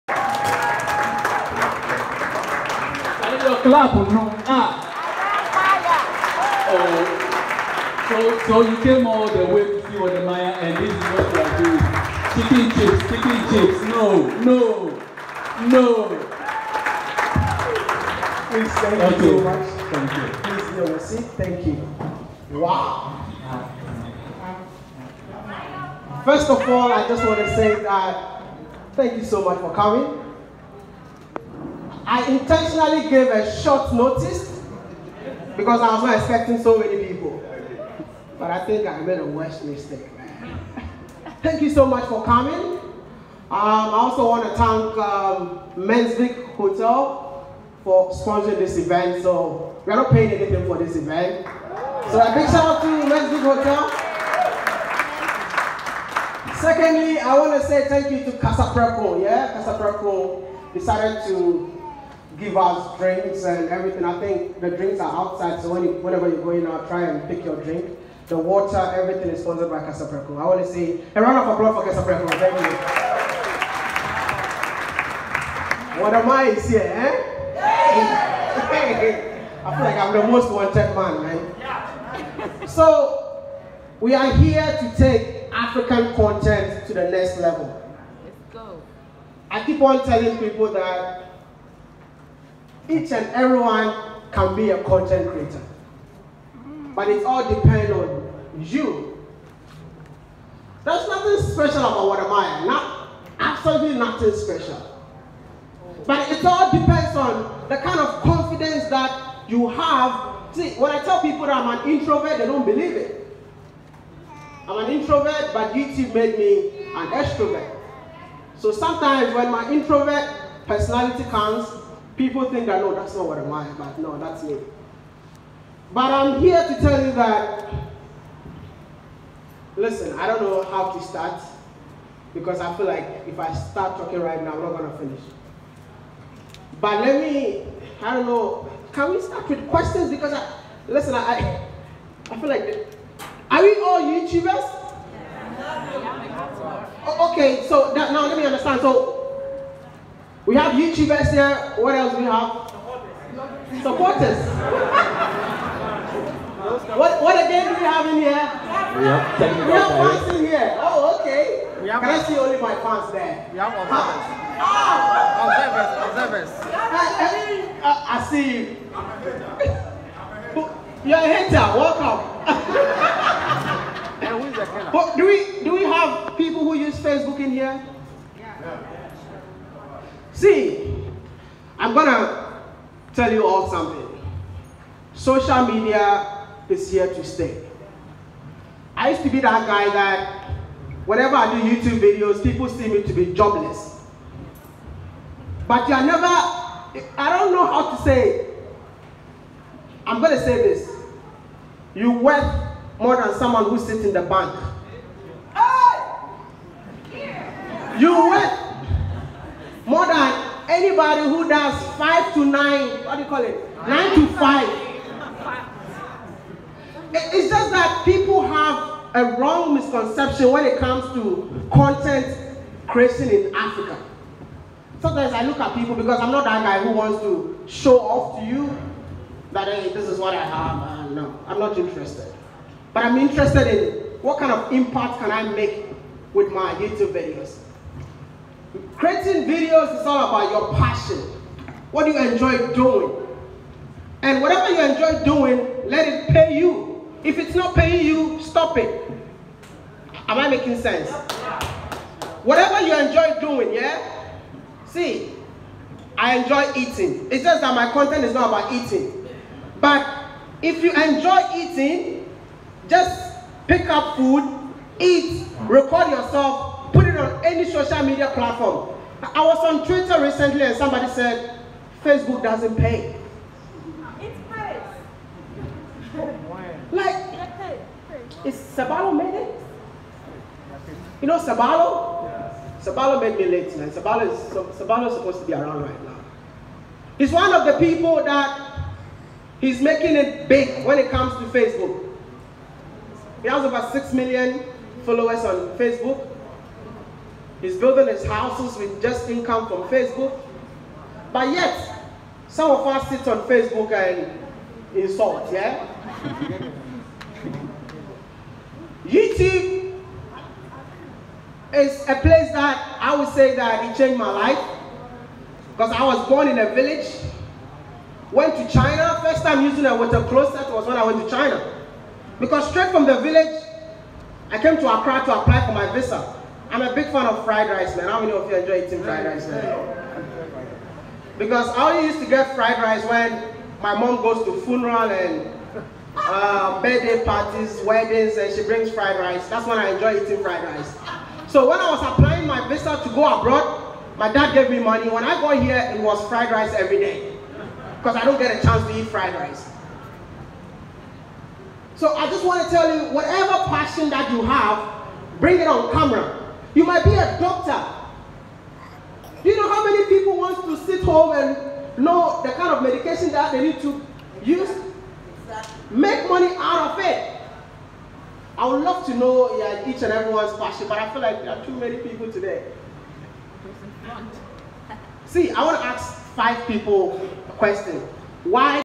Wode Maya Shares His Top Secret of Making Billions of Dollars On YouTube With Gh Content Creators
Wode maya meet and create